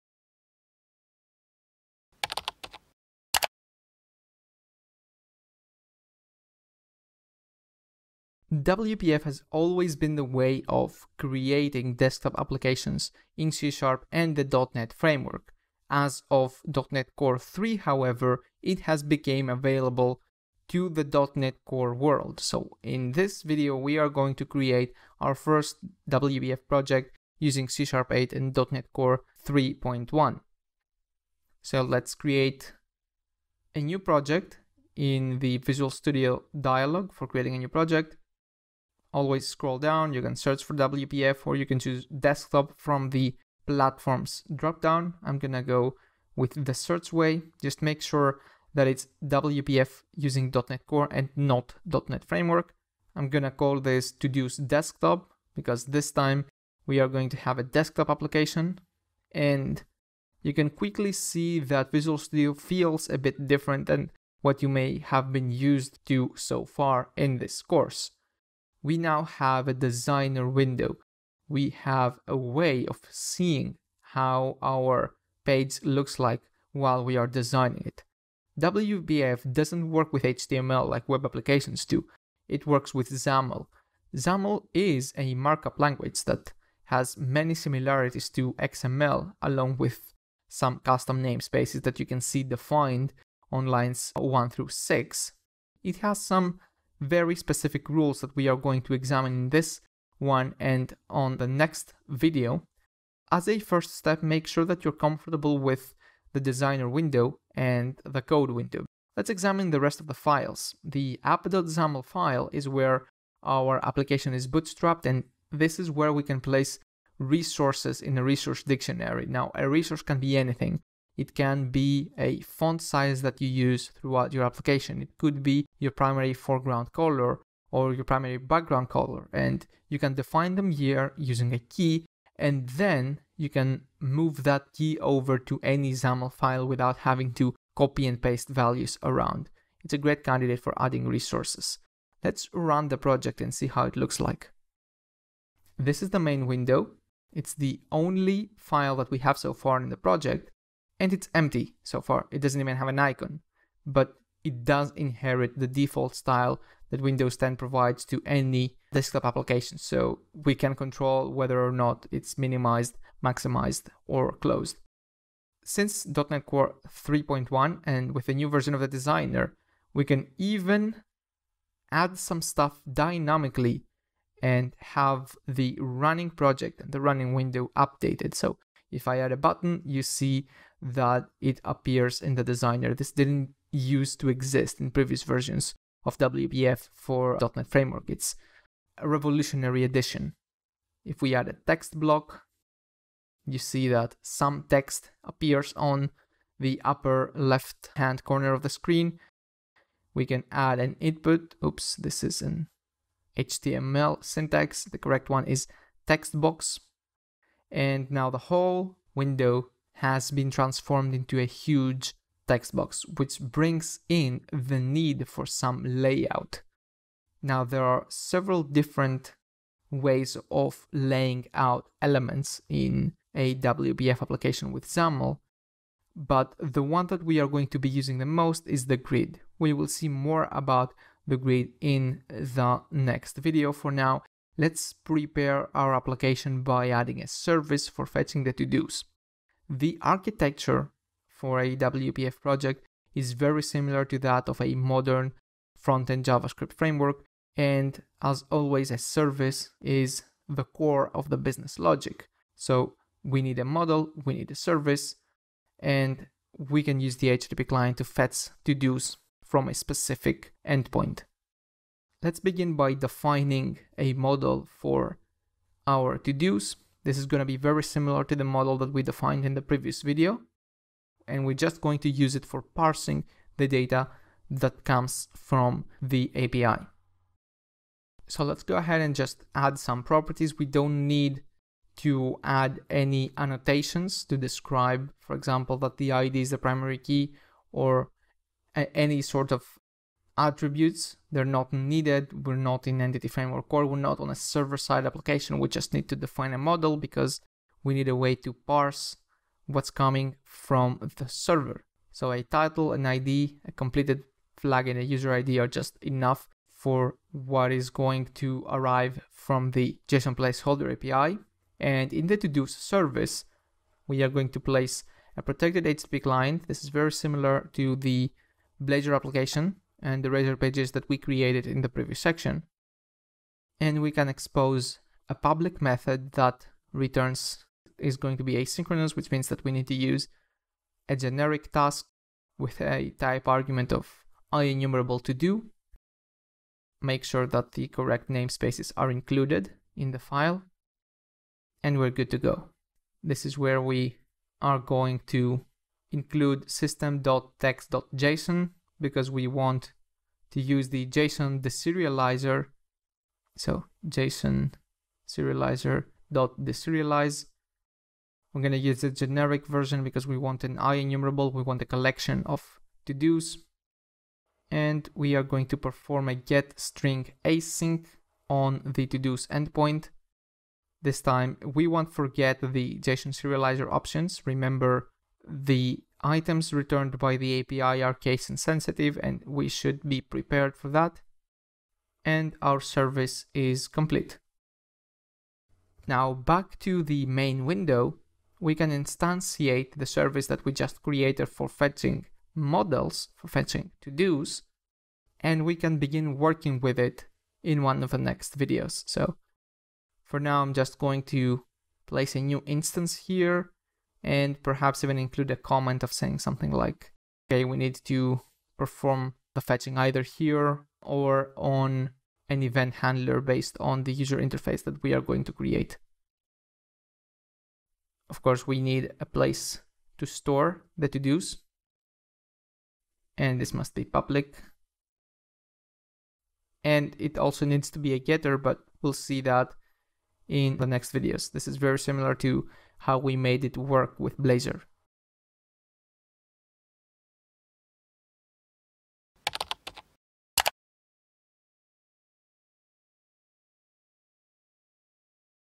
C# 8 & .NET Core 3.1 Recipes - 2nd Edition: Creating a WPF Project | packtpub.com
This video tutorial has been taken from C# 8 and .NET Core 3.1 Recipes (2nd Edition). You can learn more and buy the full video course